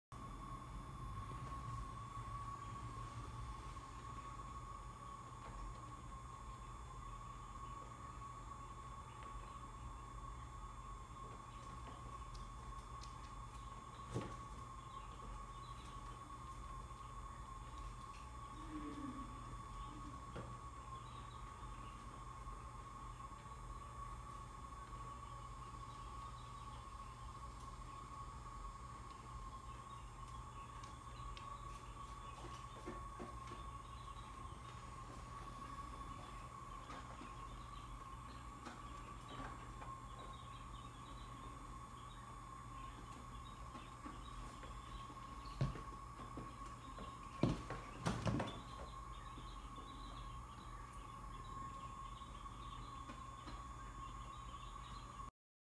White Quail walking backwards
Quail walking / running backwards